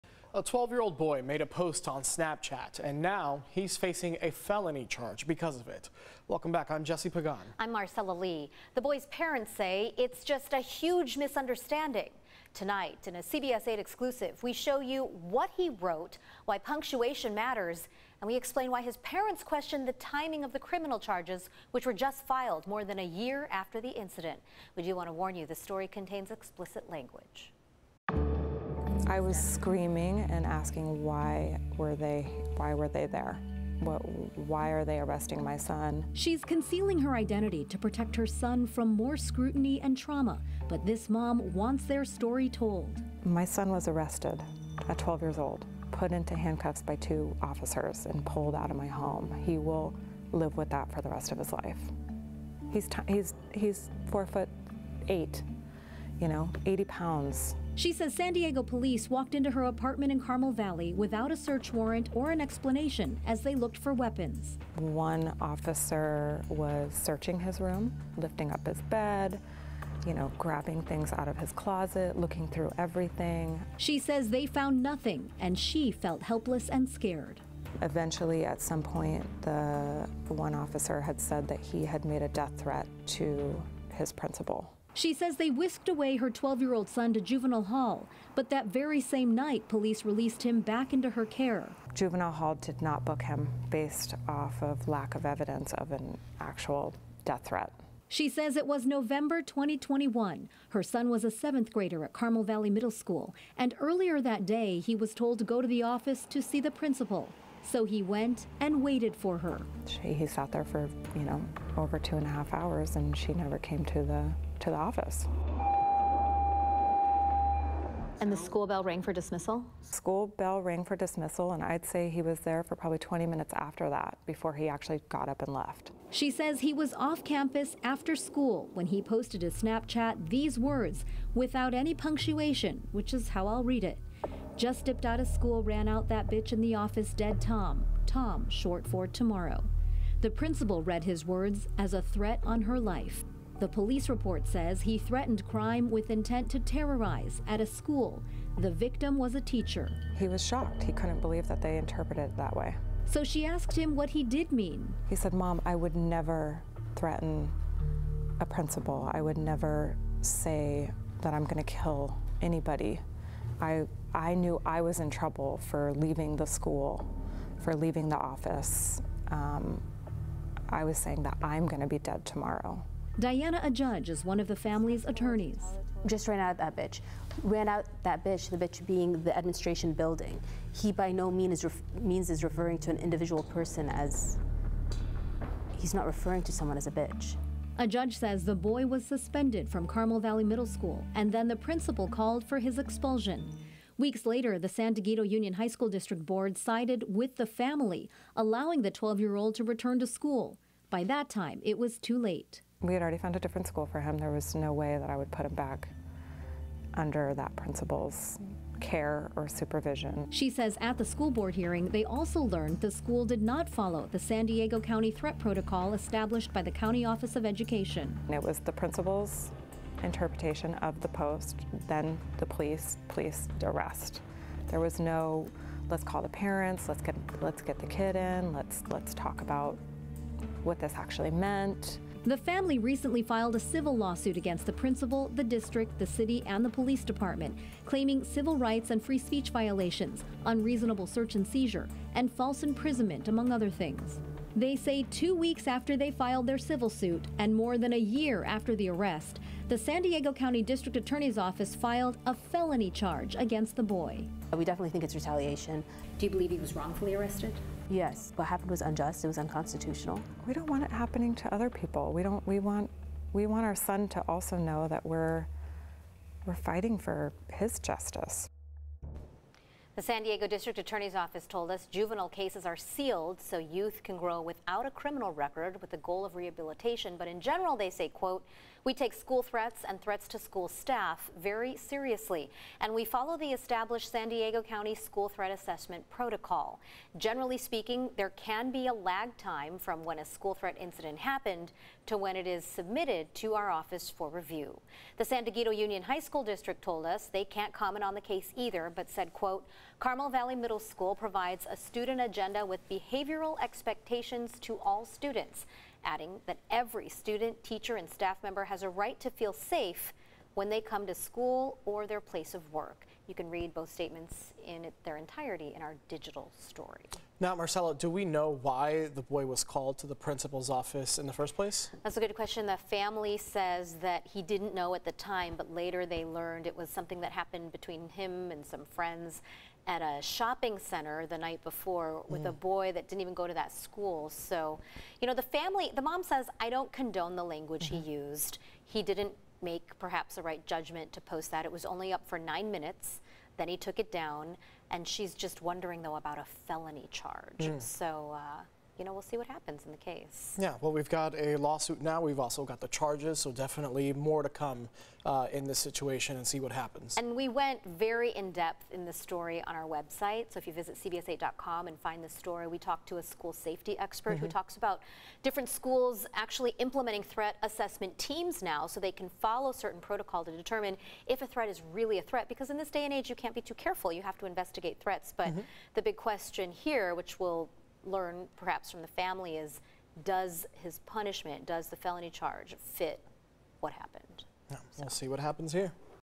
San Diego District Attorney presses felony charge against 12-year-old over Snapchat message
The 7th grader ran out of the principal's office at Carmel Valley Middle School. His 13-word Snapchat post later that day would eventually lead to a felony charge.